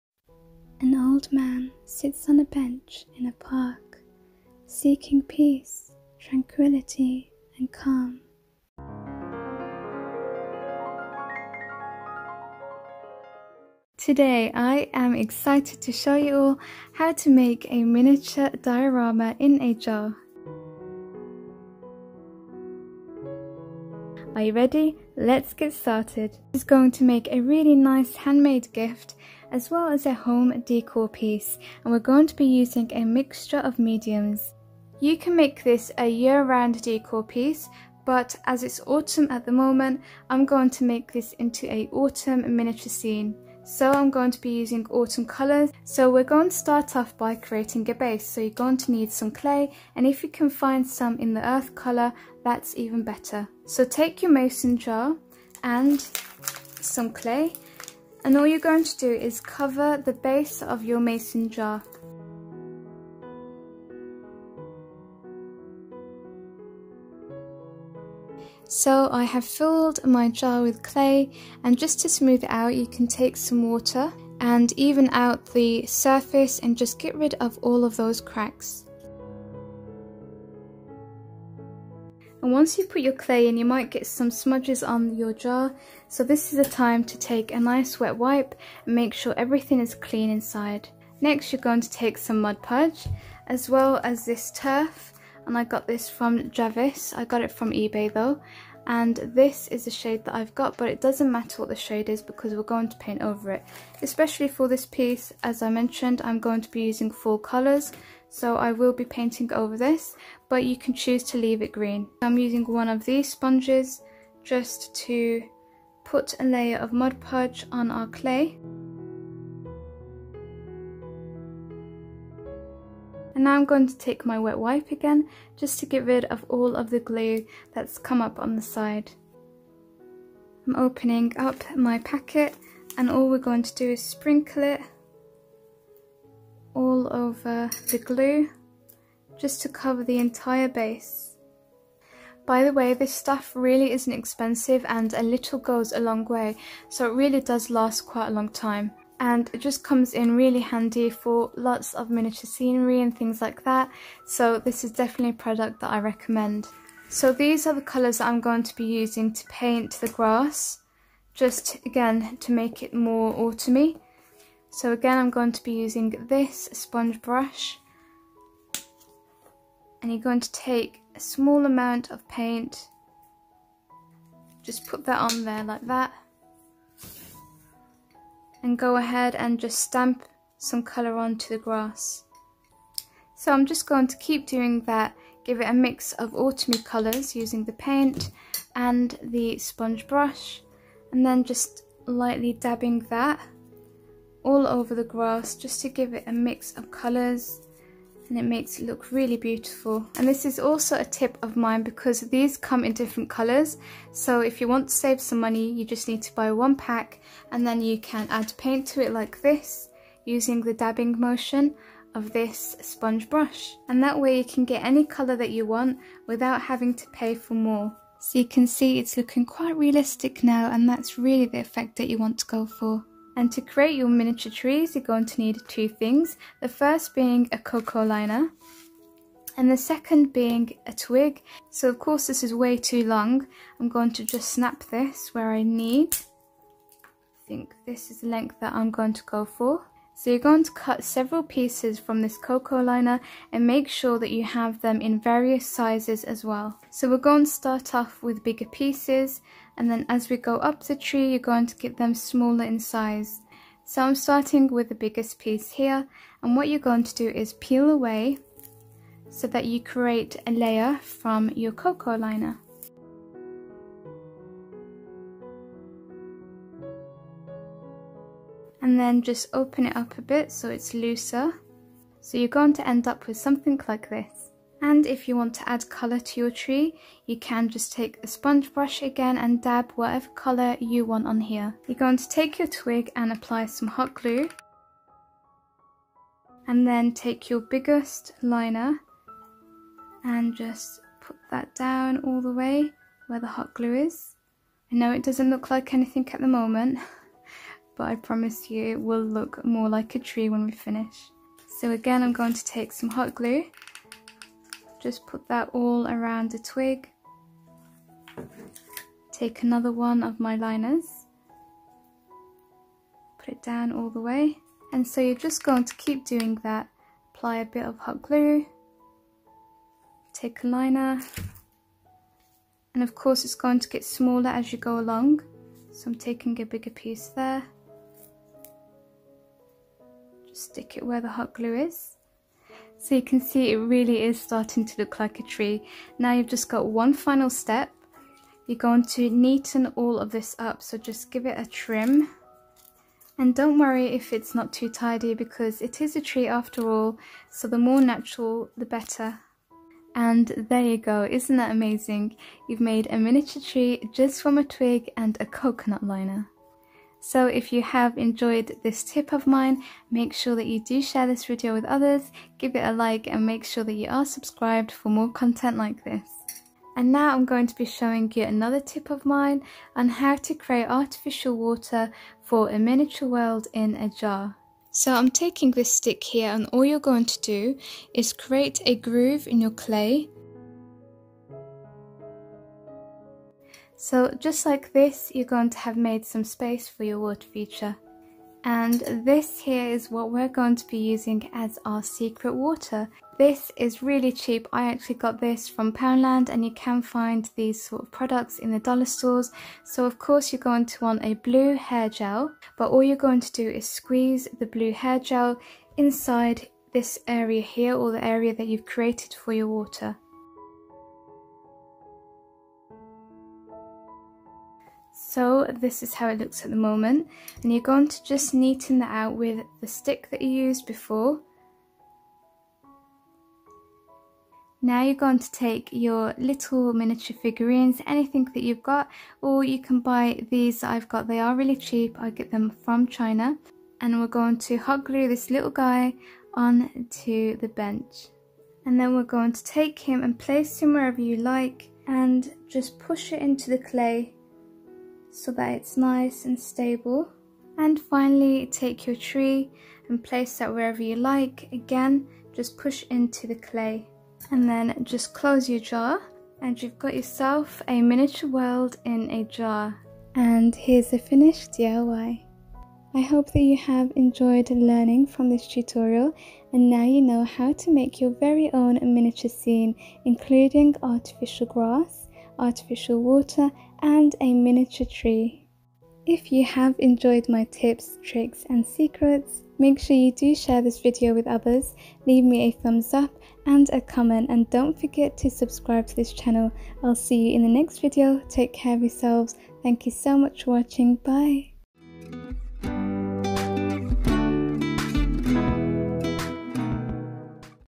DIY Miniature Garden Diorama in A Jar *with Fake Water & Grass!* Fall / Autumn Decor Craft Ideas.
In Today's video you will learn how to make a diorama or a miniature scene in a mason jar. You will also learn how to make fake water, how to make fake grass, and how to make a miniature tree! I also share a tip on how I save money.